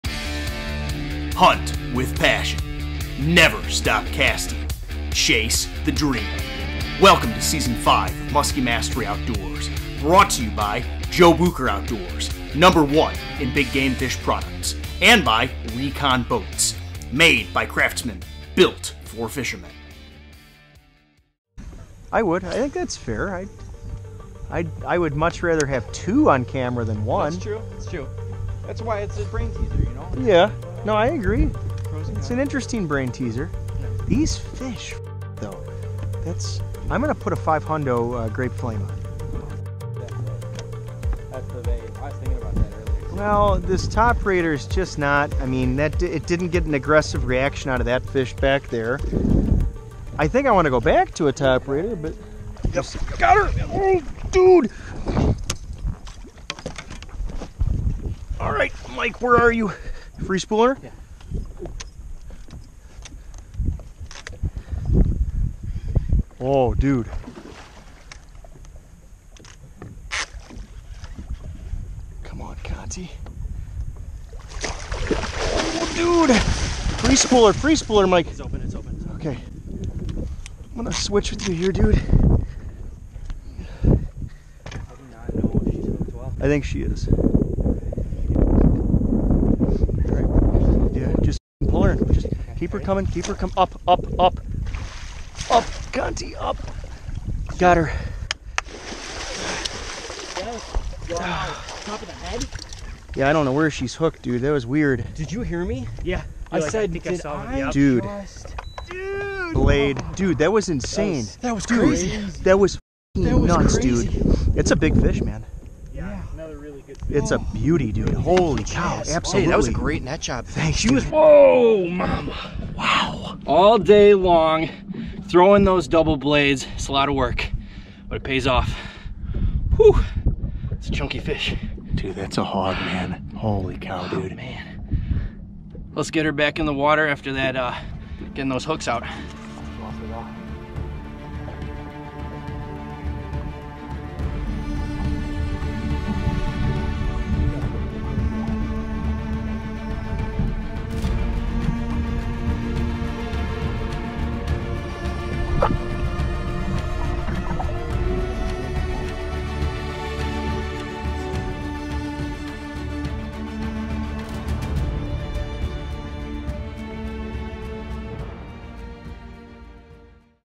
Early Fall GIANTS! BIG MUSKIES on BIG BLADES! S5.E25
As the photoperiod begins to change and the water temperatures cool the BIG muskies begin to move shallow and put the feedbag on!  The early fall period is a very special time of year during the musky season because weed growth is peaked, or just beginning to deteriorate, and these structures are holding the maximum amount of forage possible.  And of course, the muskies will be keying in on this special opportunity.  The transition from summer into fall heightens a feeding response from larger muskies that typically spend the chunk of their season suspended and now, these tankers move in shallow and feed during a special window.  Check out this video of pro staffer Michael Conti and he hooks into a total brute in the figure-8 on a Buchertail magnum tinsel.  The fight is just awesome!!!